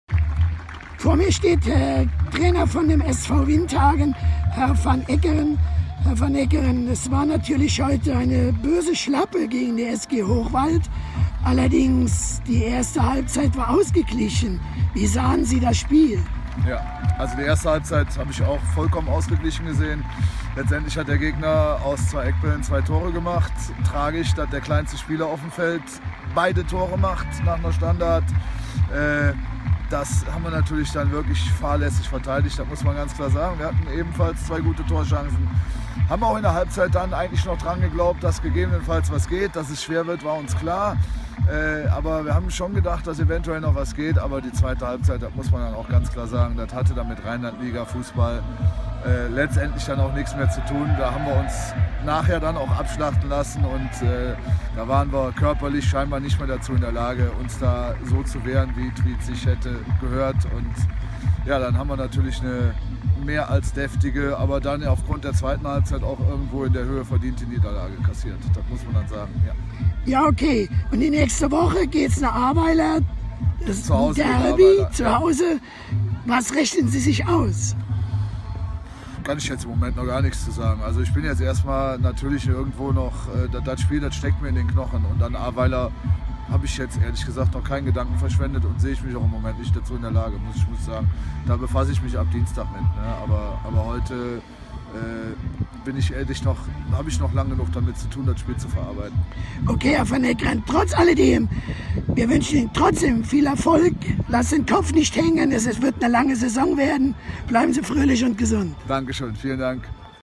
Uwe van Eckeren, Trainer SV Windhagen, nach dem 0:7 in Hentern
Video von Michael Burg